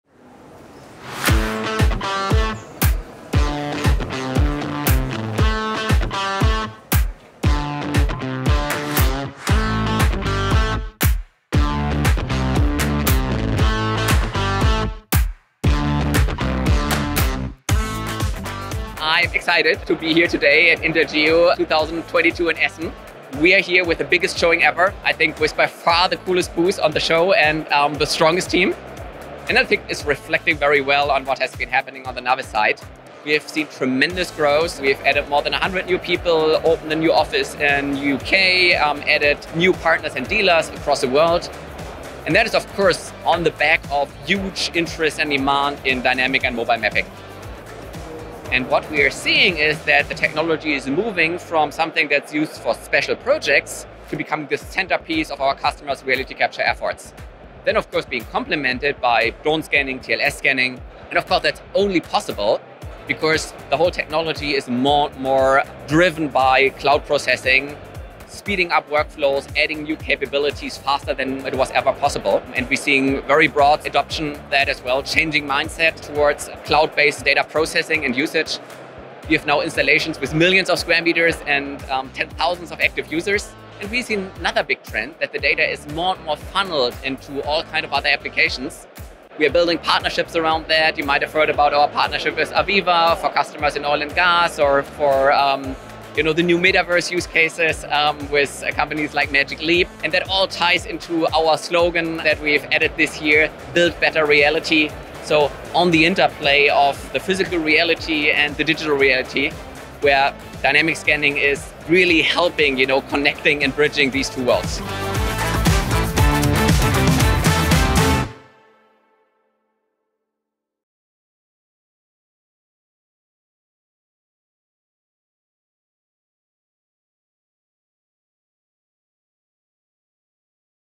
NavVis at Intergeo 2022 | Essen, DE
"We're seeing that mobile mapping technology is moving from something that is used for special projects to the centerpiece of our customers' reality capture efforts" - and this year's INTERGEO Expo & Conference confirmed that!

Watch our recap of INTERGEO 2022 with an interview from our co-founder & CEO, Felix Reinshagen. After three days of conversations, connections, and celebration on the show floor and beyond, we cannot wait for INTERGEO23!